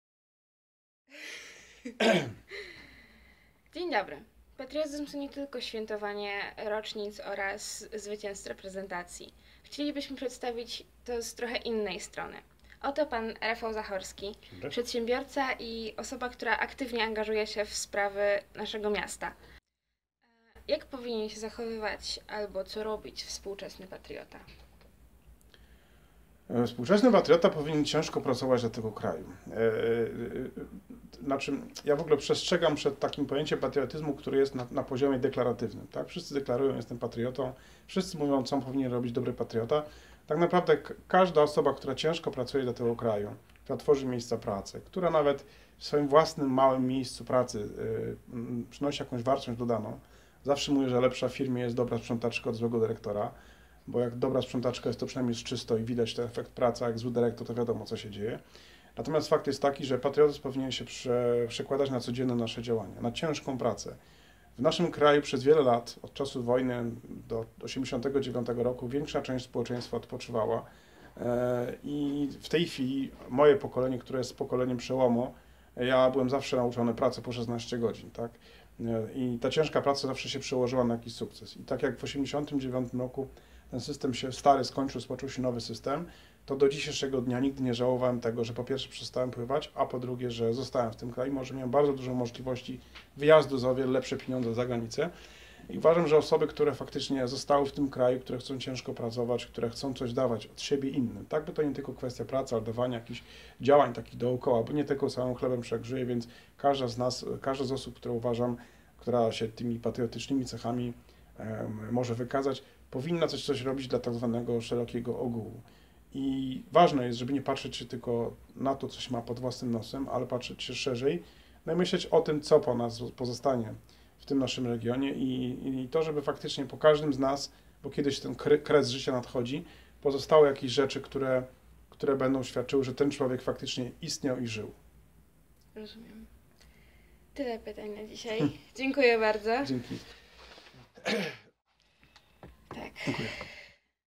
V Liceum Ogólnokształcące im. Adama Asnyka w Szczecinie
„Nowoczesny patriota w mojej okolicy.”
Filmik autorstwa uczniów V Liceum Ogólnokształcącego im. Adama Asnyka w Szczecinie. Projekt został zorganizowany w ramach Akademii Nowoczesnego Patriotyzmu. Edycja 2015/2016.